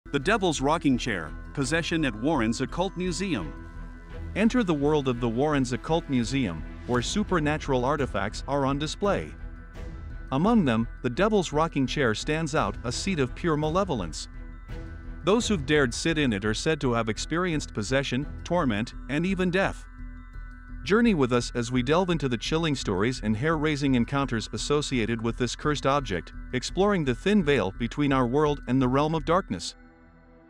👿 The Devil's Rocking Chair Possession at Warren's Occult Museum
Get ready to delve into the chilling depths of the supernatural as we uncover the spine-tingling mystery behind the infamous Devil's Rocking Chair. 🪑👻 Join us on a journey to Warren's Occult Museum, where malevolent forces converge and tales of possession come to life.

🔍 In this hair-raising YouTube short, we unlock the secrets of the past and explore firsthand accounts of unexplainable occurrences. Witness the sinister history that shrouds the Devil's Rocking Chair and learn about the spine-chilling events that have left even the bravest souls trembling. From eerie whispers to inexplicable movements, you won't believe what we've uncovered!

🕵️‍♂️ Our investigation will take you through the dark corridors of the occult museum as we piece together the terrifying puzzle of possession. Whether you're a seasoned paranormal enthusiast or a curious spectator, this video is your VIP ticket to an unforgettable encounter with the unknown.

🔮 Prepare to be captivated by the stories that have sent shivers down the spines of those who dare to cross the threshold. Don't miss out on this bone-chilling exploration of the Devil's Rocking Chair and its unsettling history of possession. 💡👁️‍🗨️